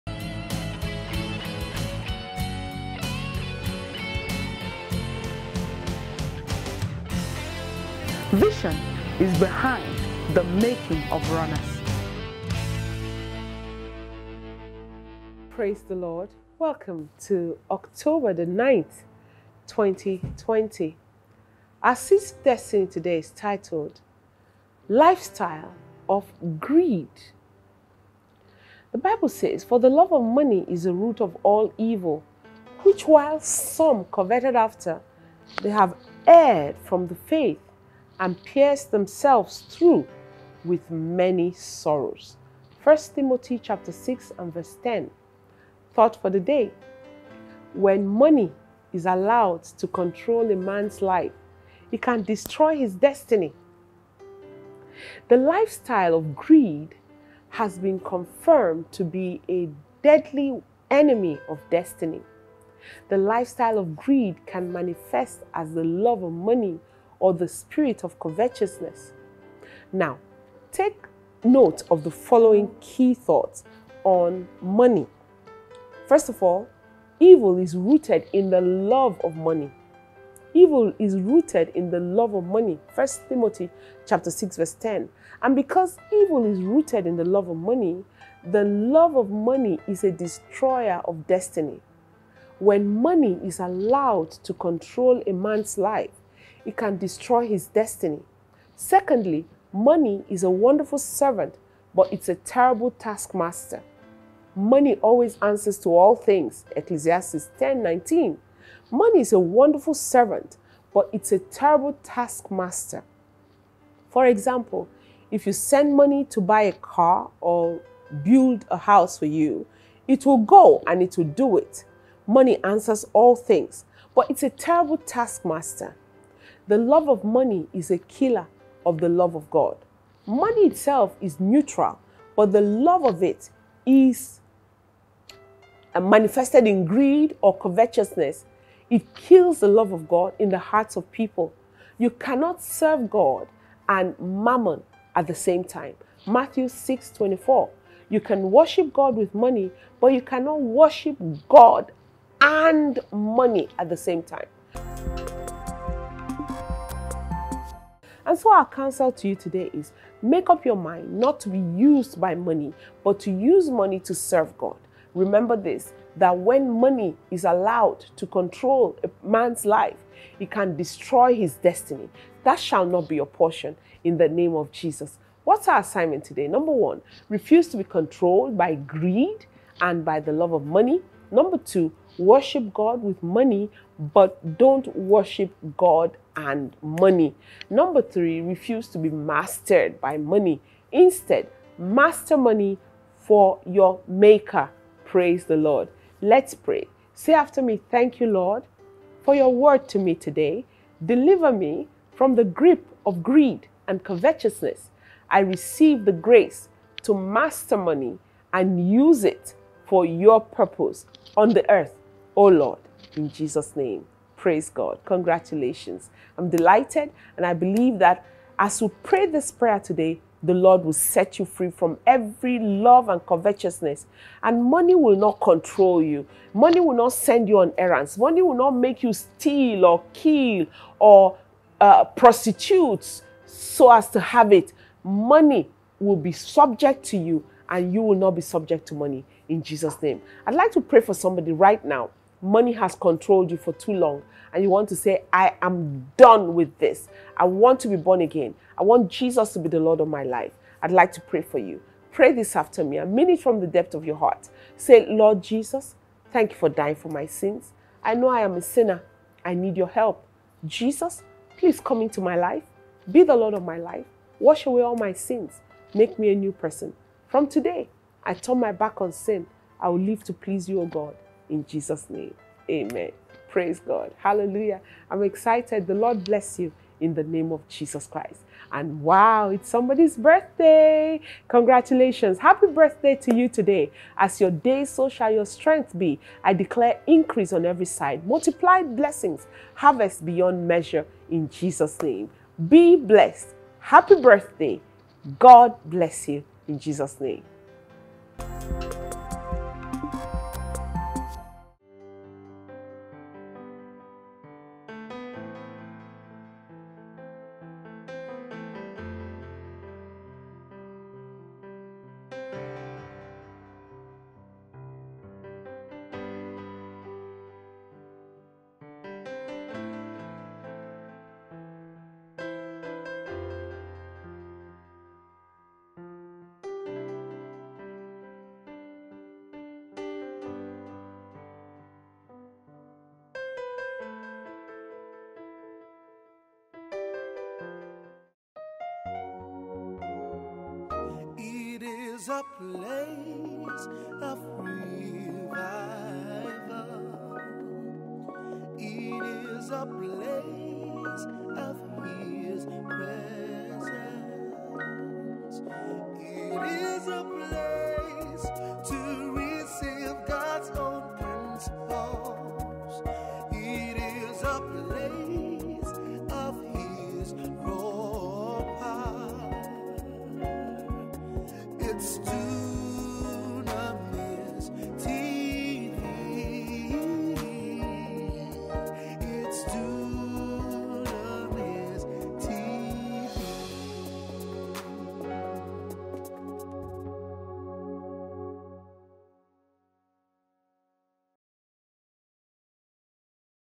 Dr Becky Paul-Enenche - SEEDS OF DESTINY - FRIDAY OCTOBER 9, 2020
TOPIC: THE LIFESTYLE OF GREED
You can worship God with money but you cannot worship God and money. Click to WATCH today’s